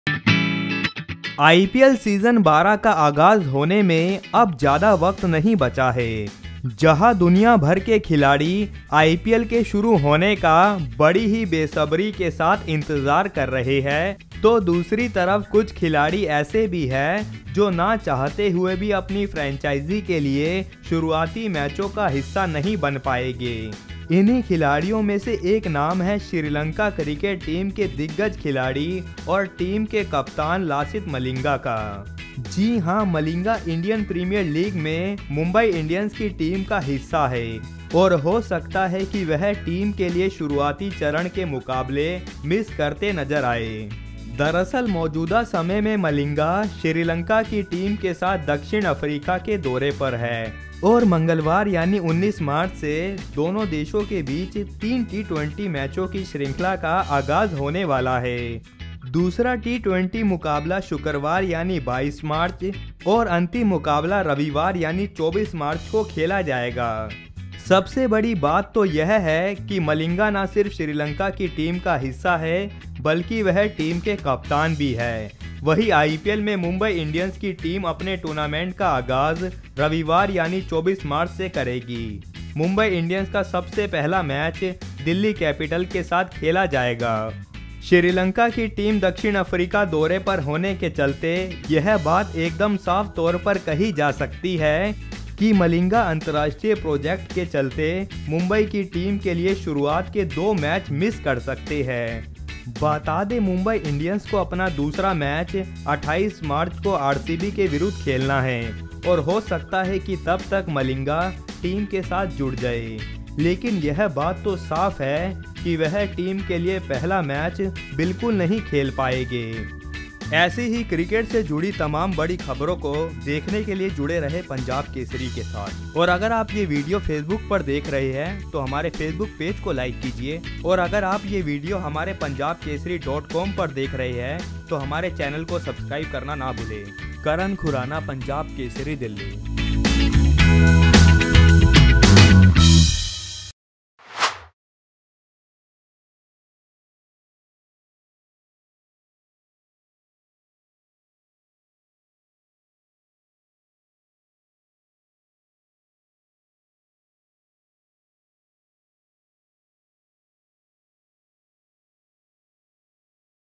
IPL 2019: Mumbai Indians के लिए शुरूआती मैचों में नहीं खेलेगे Lasith Malinga | Cricket Kesari
आईपीएल के 12वें सत्र का आगाज होने में अब ज्यादा वक़्त नहीं बचा हैं. बहुत ही जल्द विश्व क्रिकेट की सबसे बड़ी और लोकप्रिय ट्वेंटी-20 प्रतियोगिता का जोरदार आगाज होने वाला हैं. मौजूदा समय में जहाँ दुनियाभर के खिलाड़ी आईपीएल के शुरू होने का बड़ी ही बेसब्री के साथ इंतजार कर रहे हैं, तो दूसरी तरह कुछ खिलाड़ी ऐसे भी हैं जो ना चाहते हुए भी अपनी अपनी फ्रेंचाइजी के लिए शुरूआती मैचों का हिस्सा नहीं बन पायेगे.